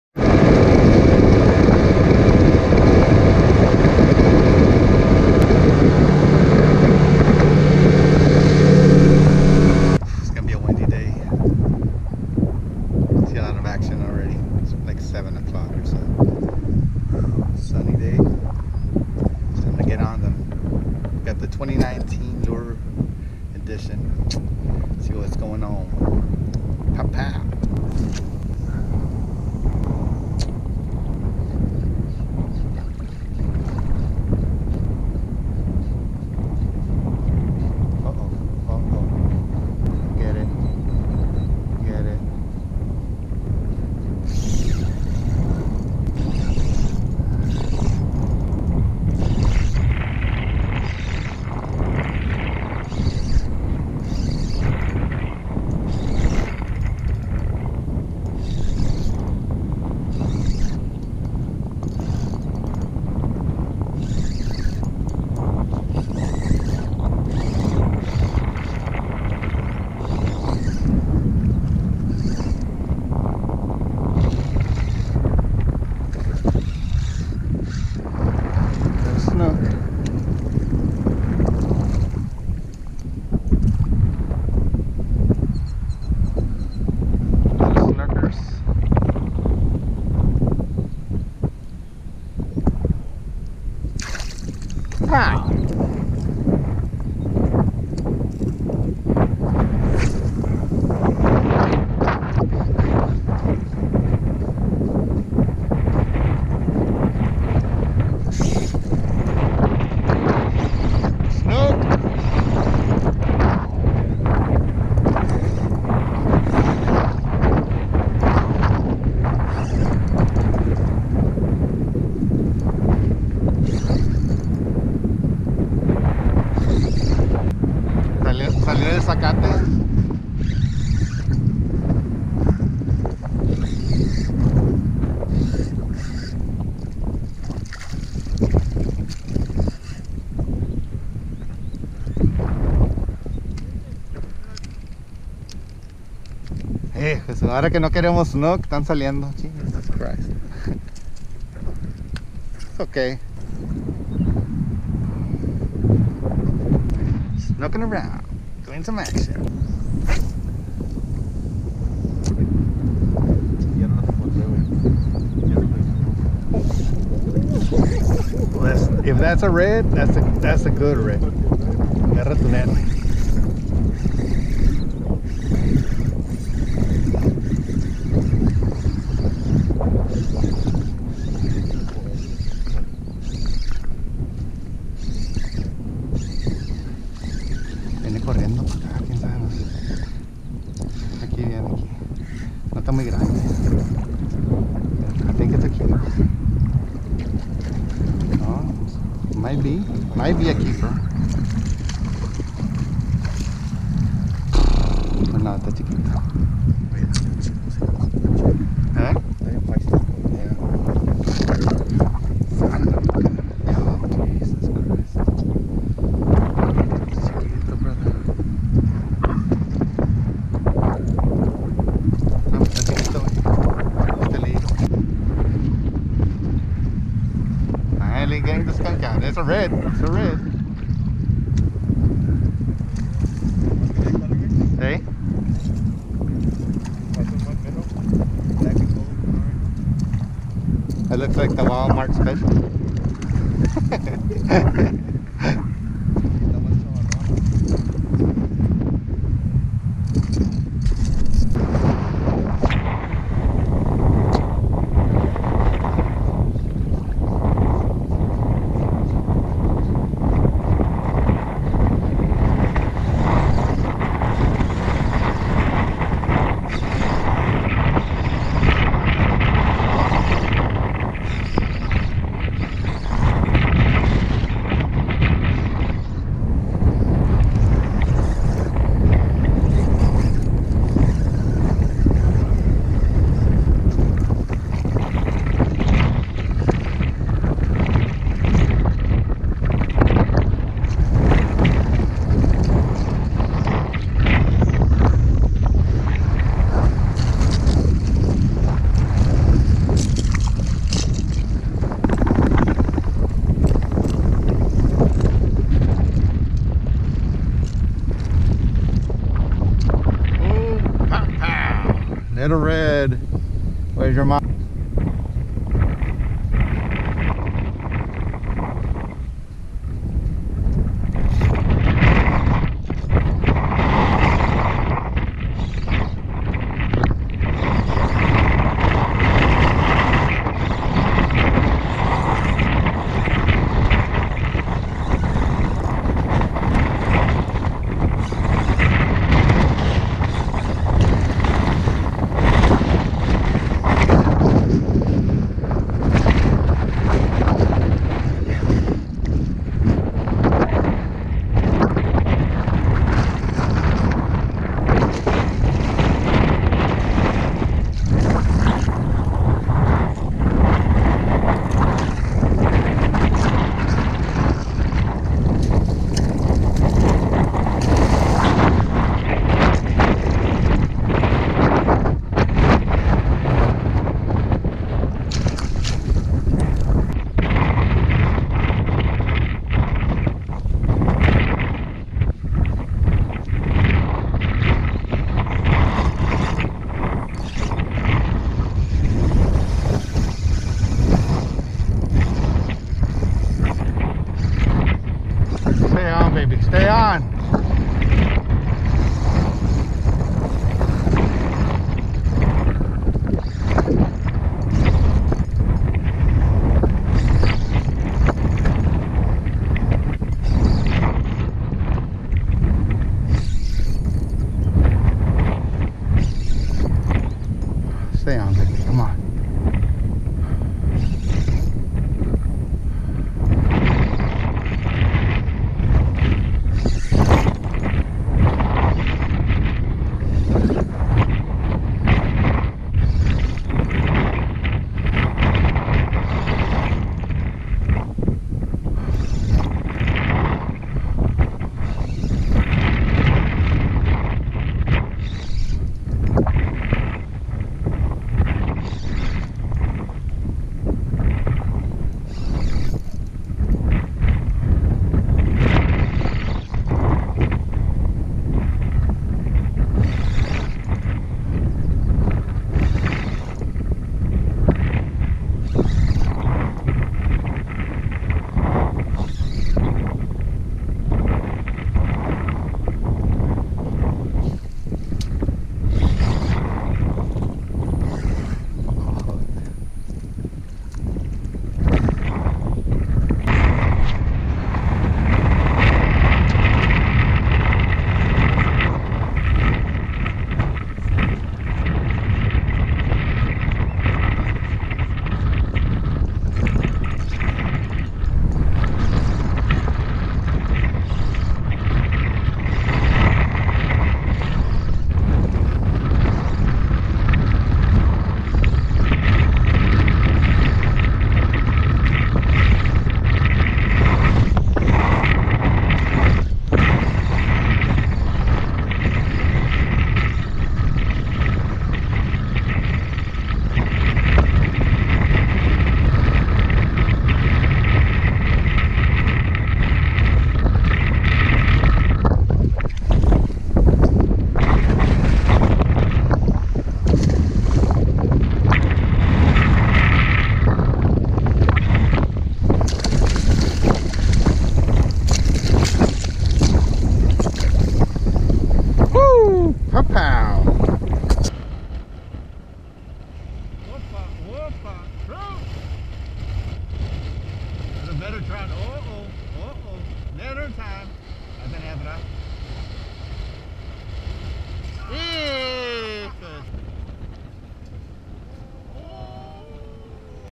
HOW TO CATCH SNOOK AT GAYMAN BRIDGE
Went on the hunt for some reds but only caught 1 keeper red and some snook...Lol funny how when we chase reds we still end up with SNOOK and hear me complain lol...Tight lines...